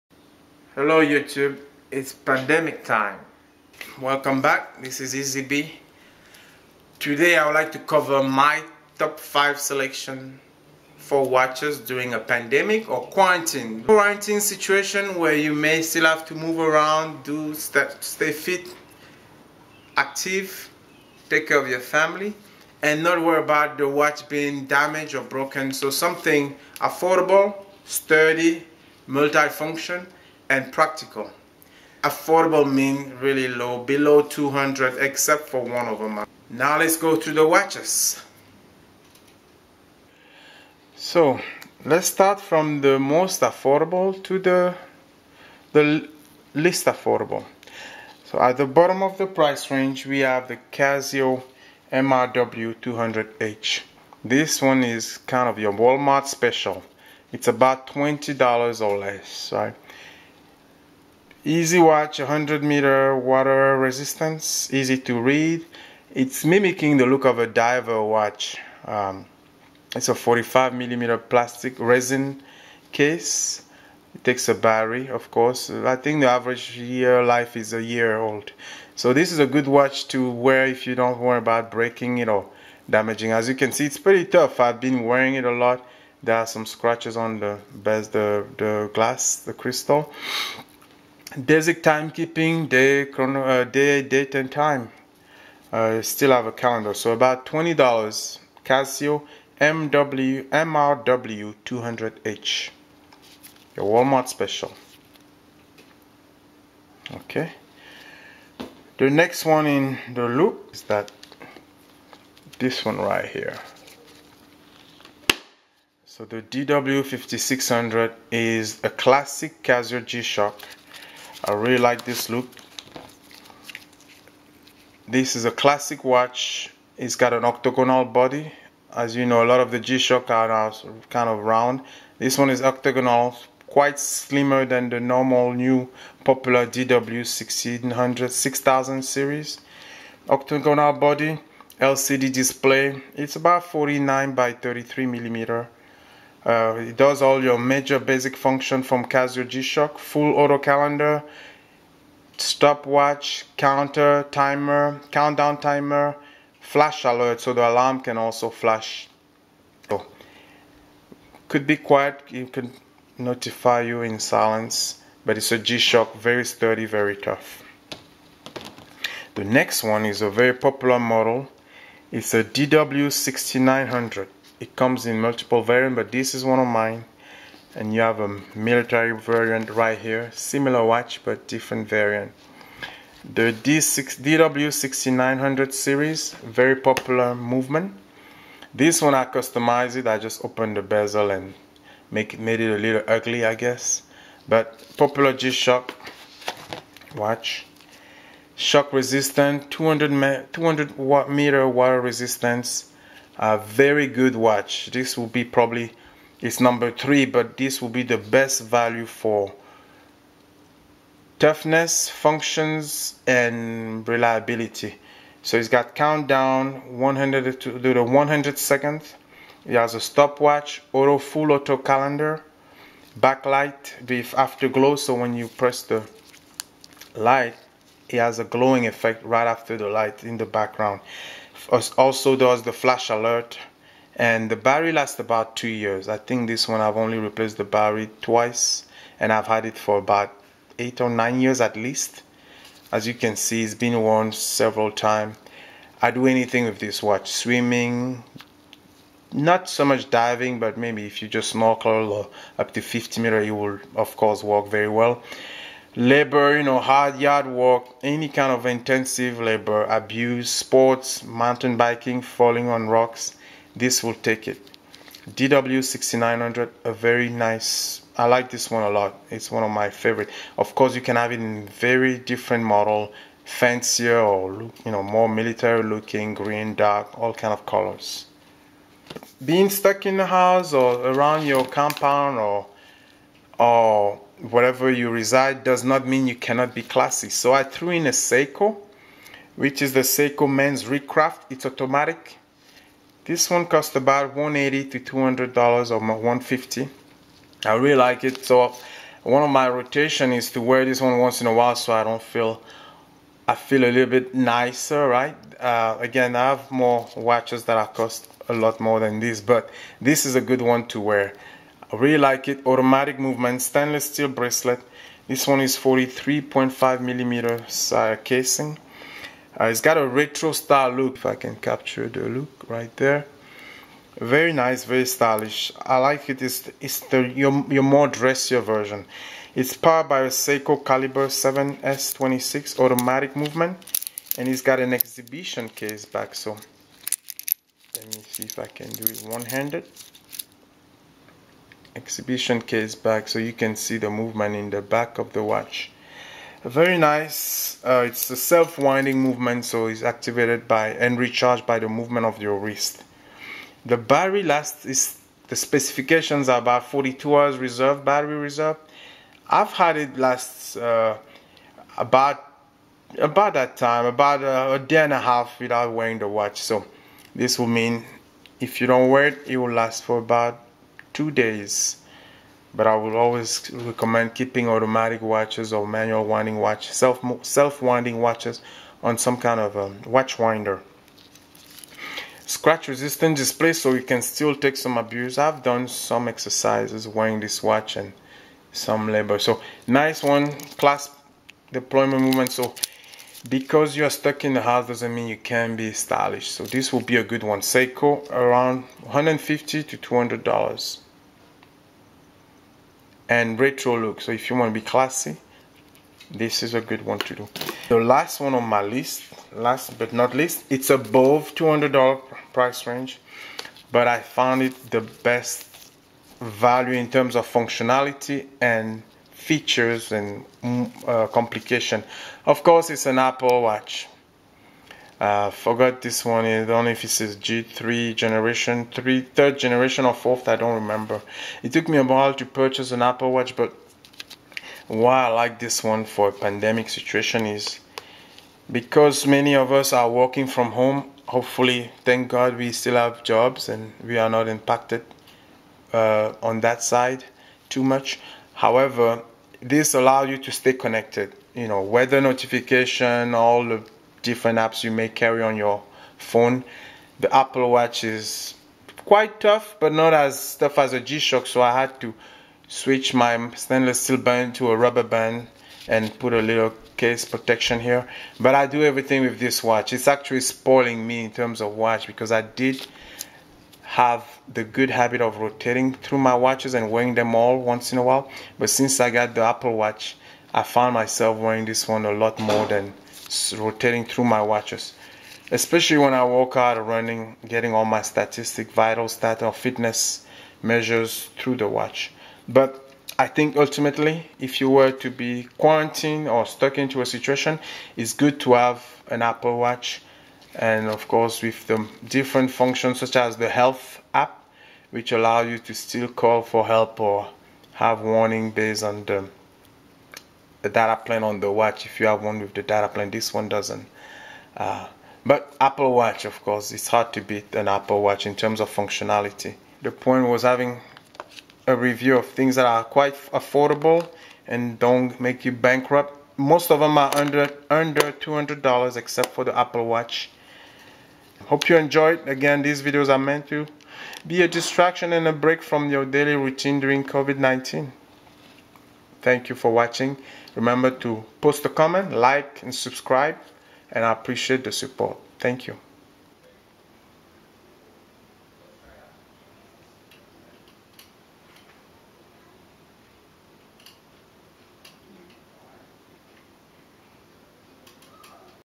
Top 5 affordable quarantine watches by EasyB
A quick review of the top affordable 5 quartz and automatic watches to wear during a quarantine period. Watches from my collection. Casio, Seiko, and Apple watches.